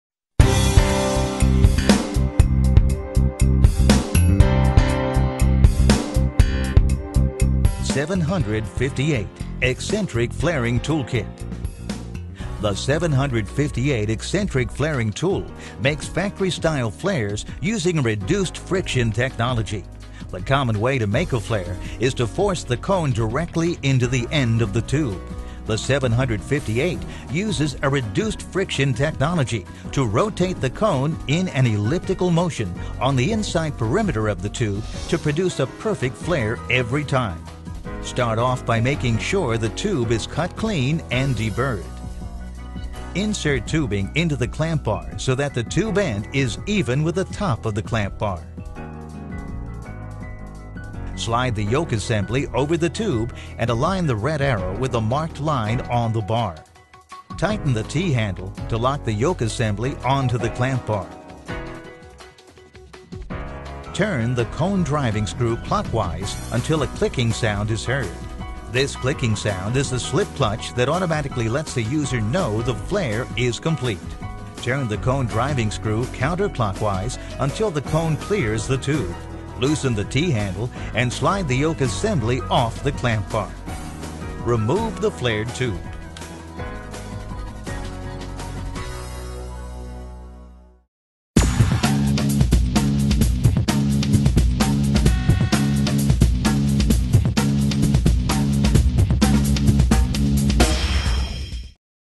קיט פלרינג לעבודה עם צנרת - Mastercool 70058
סרטון המתאר את אופן העבודה עם הכלי של חברת Mastercool המייצר צורת קונוס (פלר) לצינור הנחושת במערכת מיזוג האוויר והקירור.